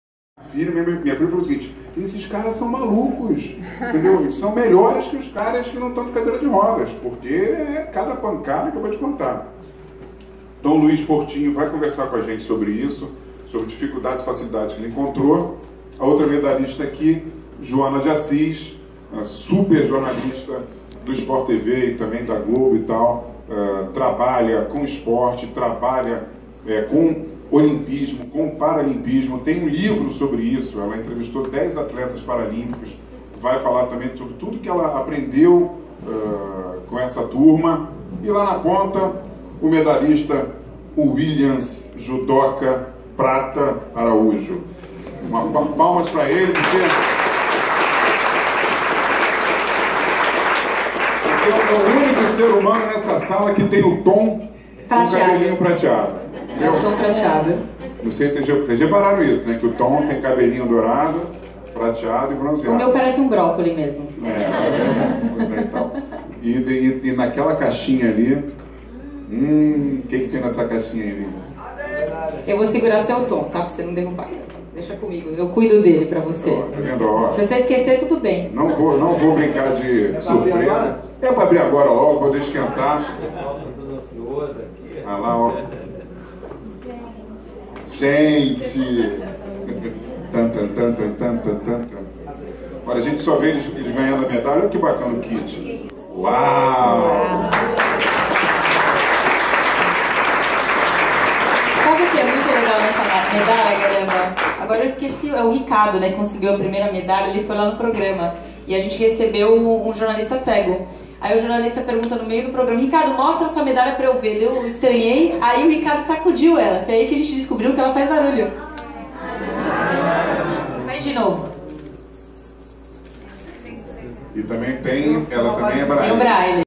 Esporte e Inclusão - Jogos Paralímpicos 2016
Presidente da RS Paradesporto Luiz Portinho participou de seminário com Joanna de Assis e Sidney Garambone da SporTv, Helena Werneck do Instituto Meta Social e Williams Araujo, medalha de prata no judô